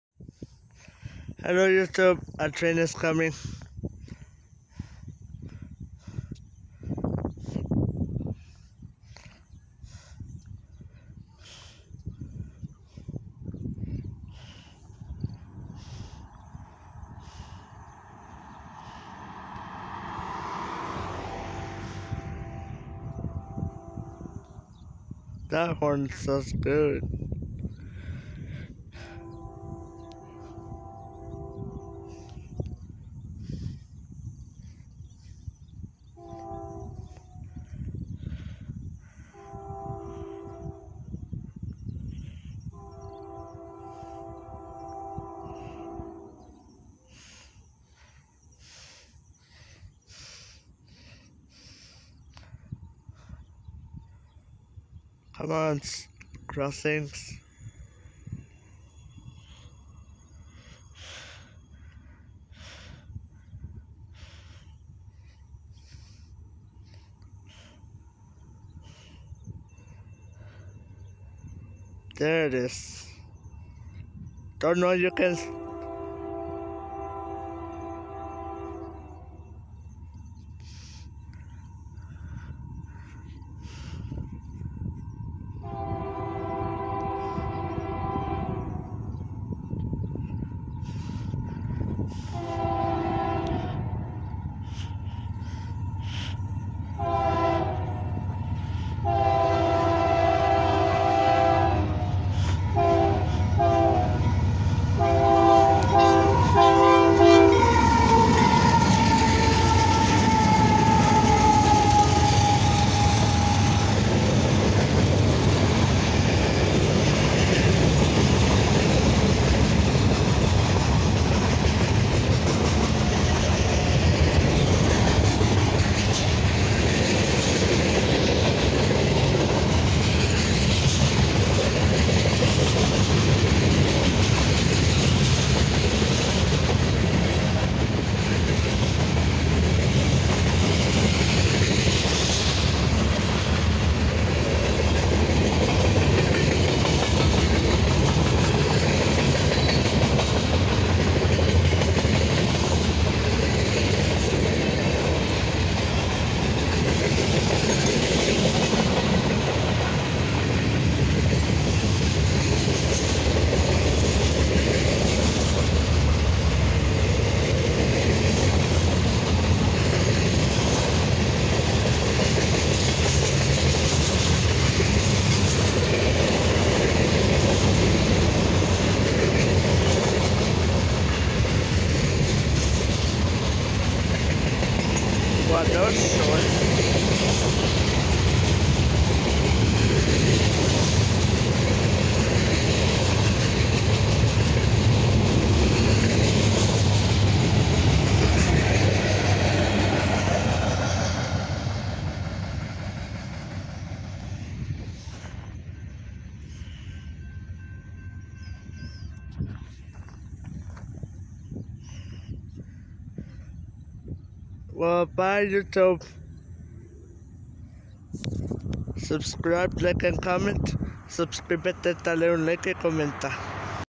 UP AC4400CW 5741 leads box cars thru EL PASO TEXAS with SH@HC horn show
UP AC4400CW 5741 (Narrow font K3LA)
UP ES44AC 2581 (Note: the cab window open)

NO MID-DPU or DPU on this train